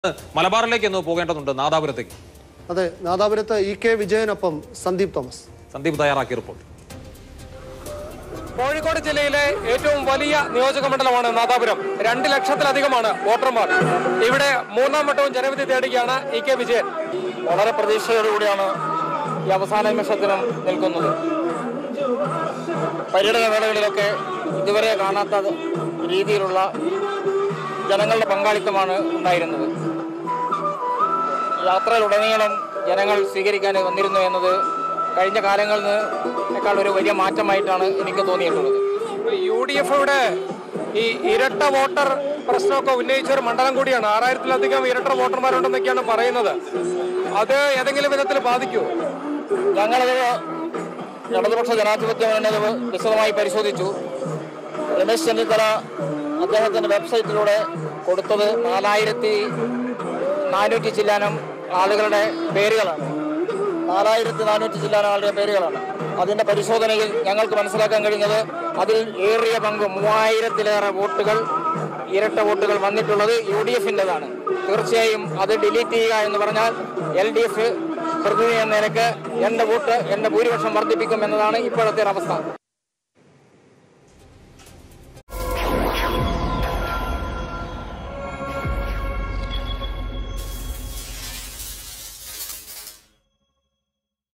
'ചെന്നിത്തല ഉന്നയിച്ച ഇരട്ടവോട്ടുകളിൽ ഏറിയപങ്കും യുഡിഎഫിന്റേതാണ്' : ഇകെ വിജയൻ | EK Vijayan
EK Vijayan about Ramesh Chennithala's dual vote allegations
ഇരട്ടവോട്ടുകൾ ഡിലീറ്റ് ചെയ്താൽ തന്റെ ഭൂരിപക്ഷം വീണ്ടും വർധിക്കുമെന്ന് നാദാപുരത്തെ എൽഡിഎഫ് സ്ഥാനാർത്ഥി ഇകെ വിജയൻ